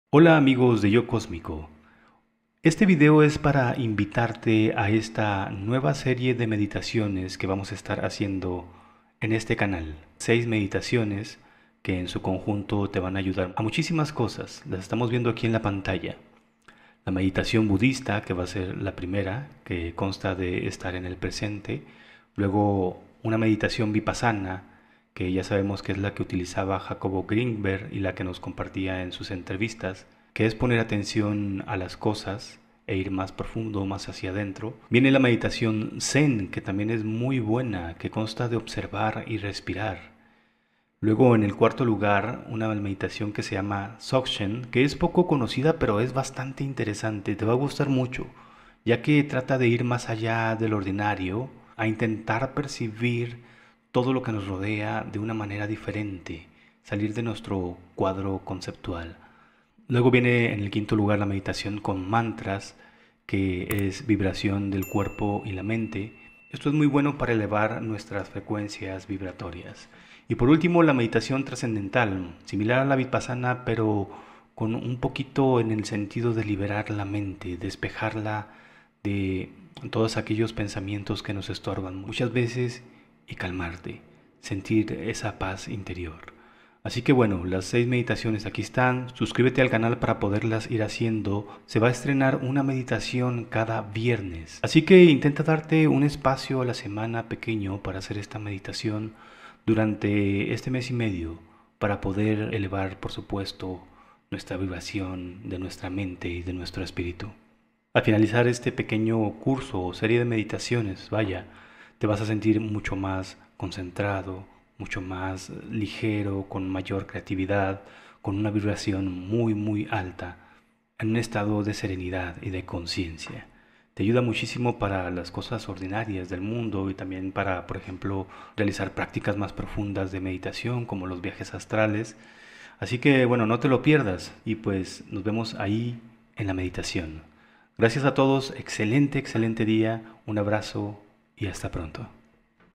Las 6 Meditaciones que Cambiaran tu Vida (Serie de vídeos Gratis)
Un grupo de 6 meditaciones que saldrán en este canal para despertar el espíritu